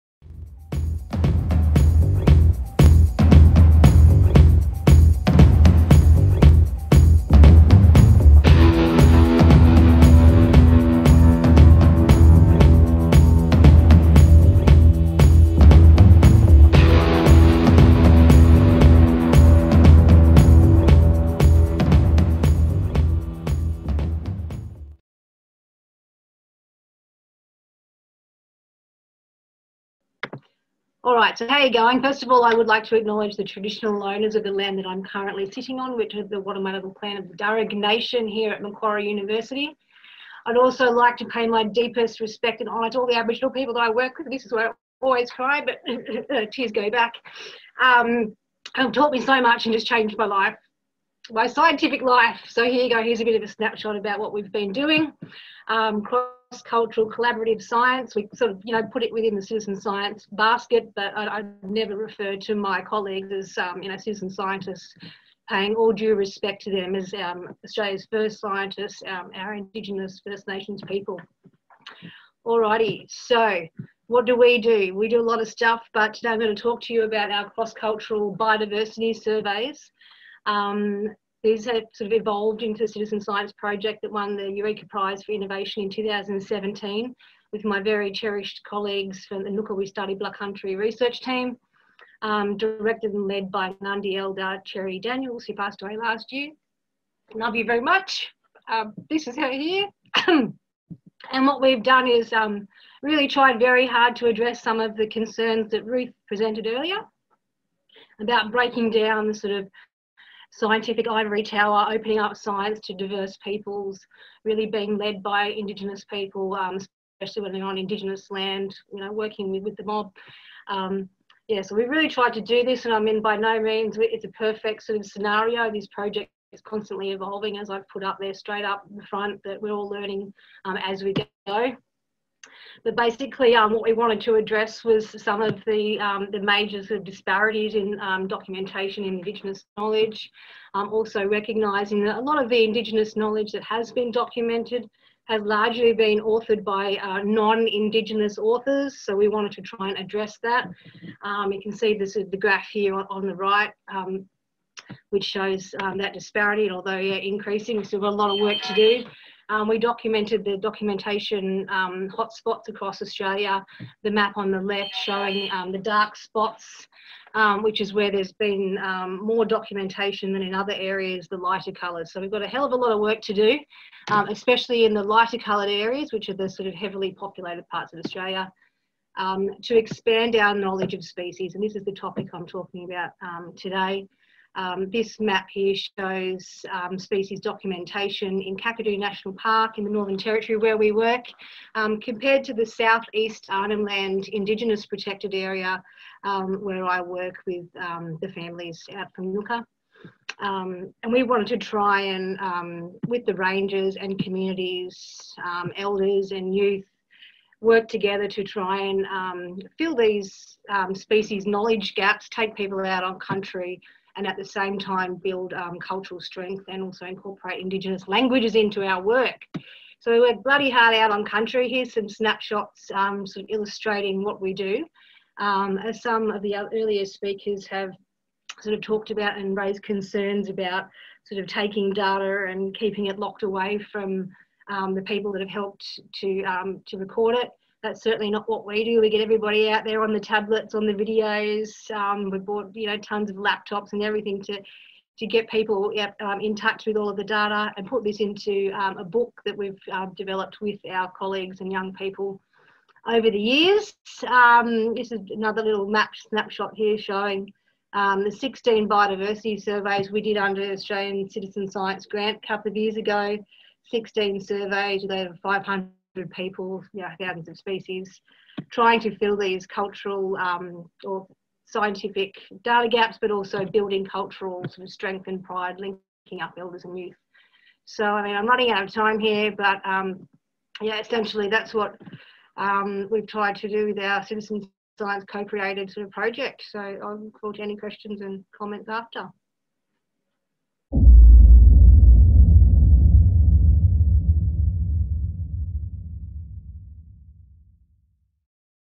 CitSciOzOnline EMCR: Cross-cultural Collaborative Science with Australia’s First Scientists - Dr Ens
Cross-cultural collaborative science with Australia’s first scientists - Dr. Emilie Ens

Aboriginal people were and are Australia’s first scientists and hence are not technically citizen scientists from their perspective. Our cross-cultural and collaborative approach to research is pushing the frontier of citizen science to consider all people as scientists, people who ask questions, and study things. Science should not be the exclusive domain of those who have a Science degree. In this talk, Dr. Ens briefly discusses the collaborative biocultural research in Arnhem land with Aboriginal Rangers, schools, and communities and how, with a focus on delivering mutual benefits, we can have outcomes for science, culture, and community development.

Dr. Emilie has worked for 12 years with Aboriginal Ranger groups and communities in Arnhem Land, and more recently Nth NSW, to co-develop cross-cultural environmental monitoring tools. She has co-authored many publications including a 46 author multilingual field guide on cross-cultural and collaborative approaches to science. With the Ngukurr Wi Stadi Bla Kantri (We Study the Country) Research Group, she won the 2017 Eureka Prize for Innovation in Citizen Science.

Twitter: @EmilieEns @MQEarthEnv @cachemq @Macquarie_Uni